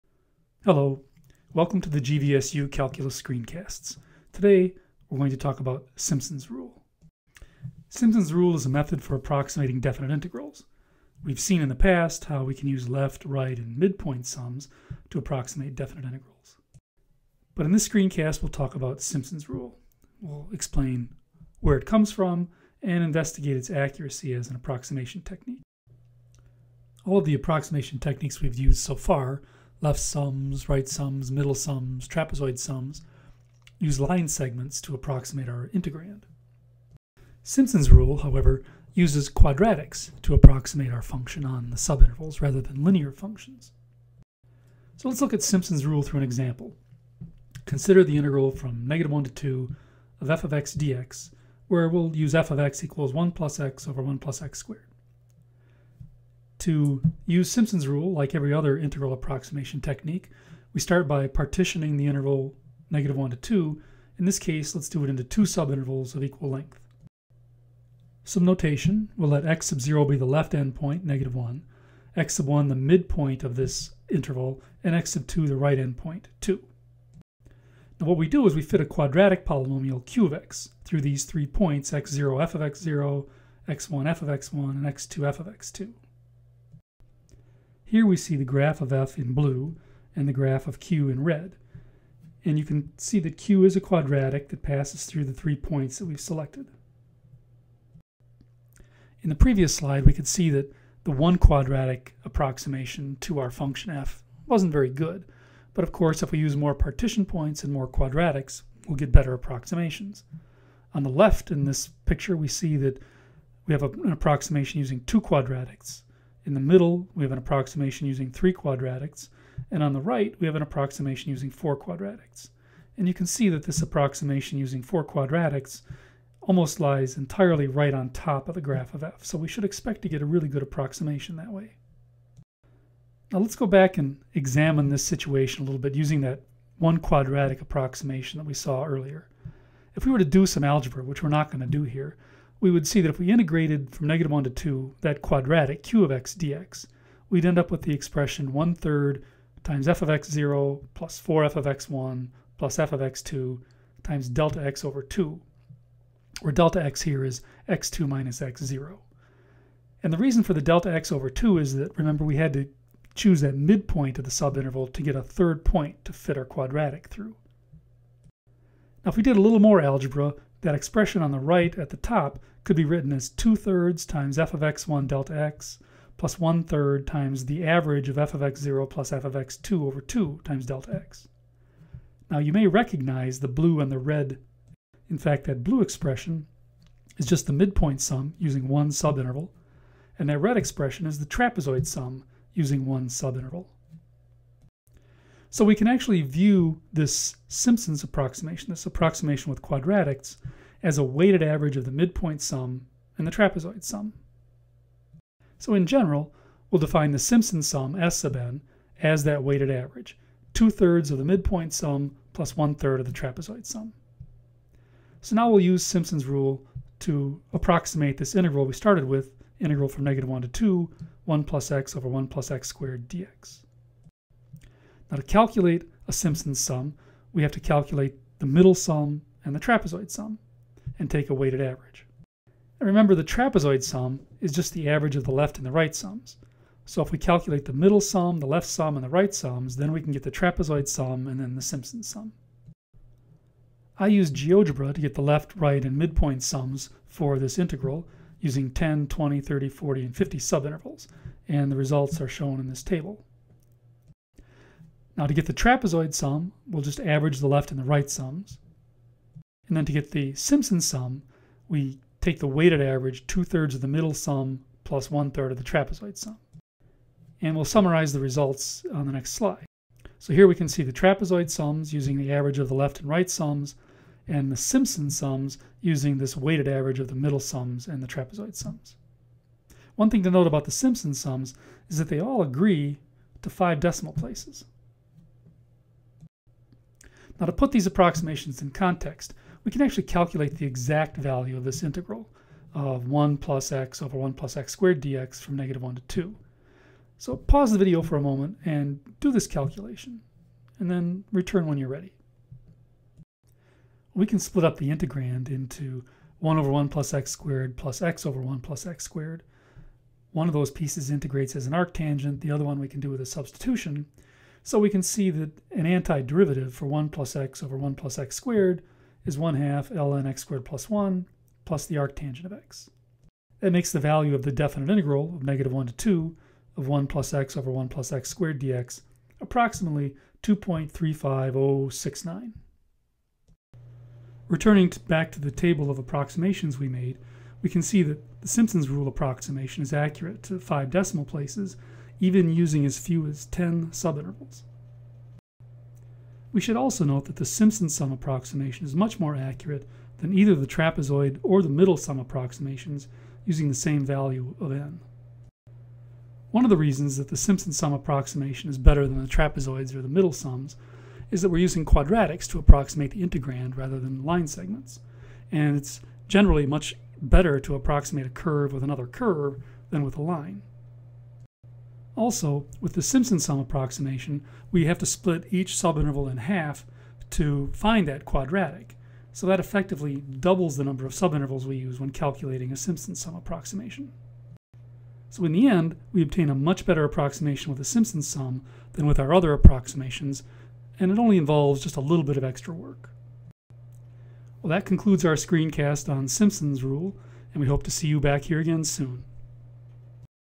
Screencast 5.6.5: Simpson's Rule
This video gives examples of using Simpson's Rule to estimate the value of an integral.